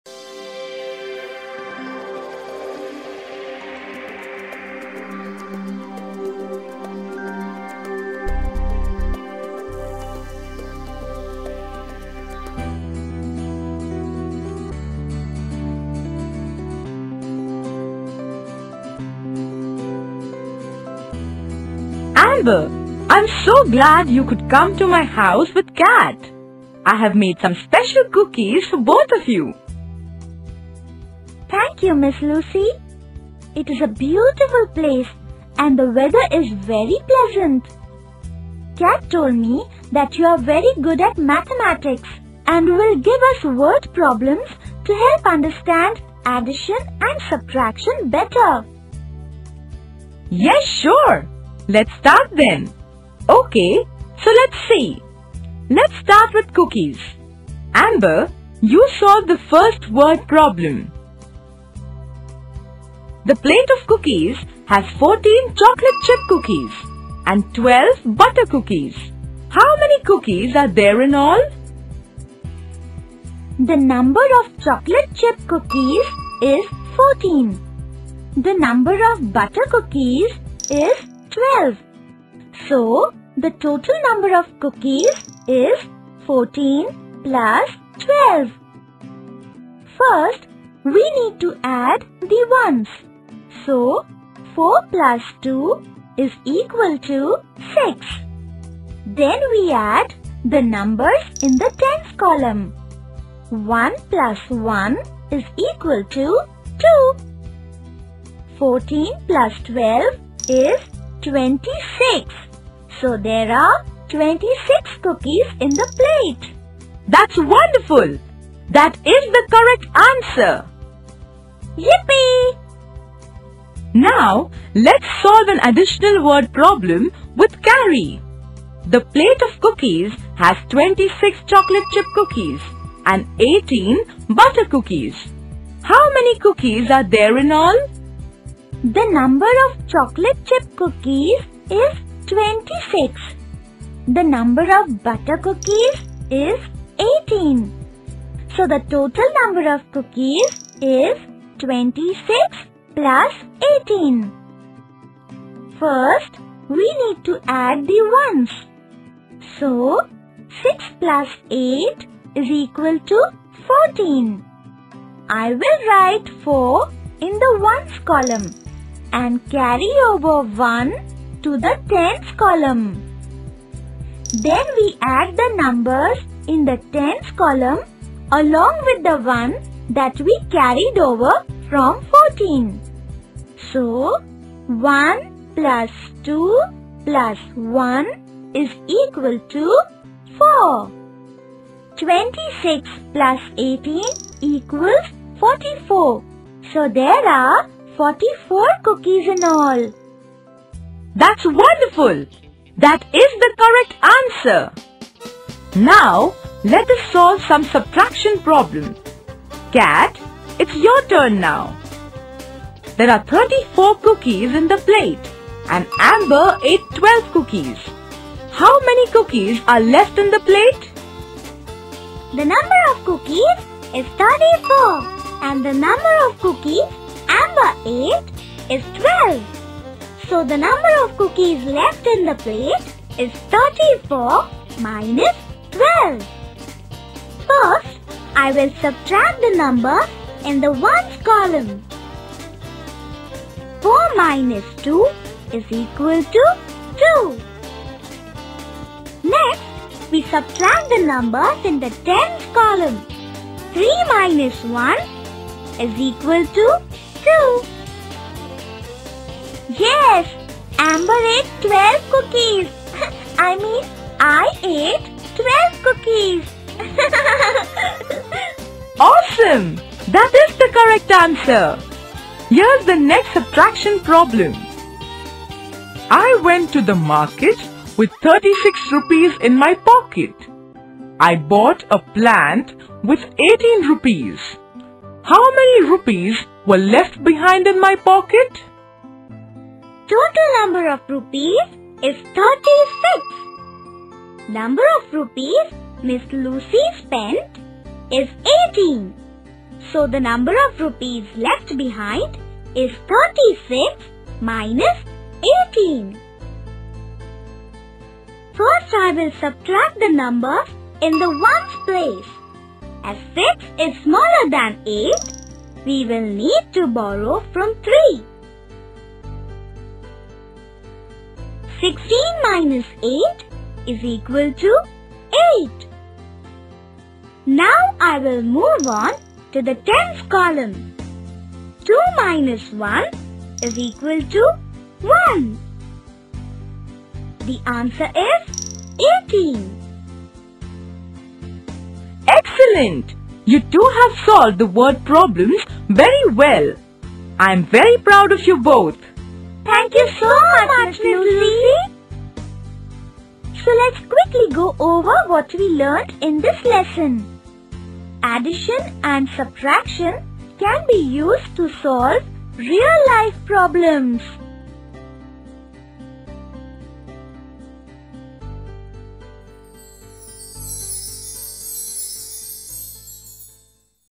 Learn Mathematics: Addition and Subtraction Word Problems
Learn mathematics by watching Education videos by Mexus Education. In this vide you will learn to solve addition and subtraction problems in easiest ways. The tips and tricks to solve these problems mentioned in this video is important for you so watch the video and learn.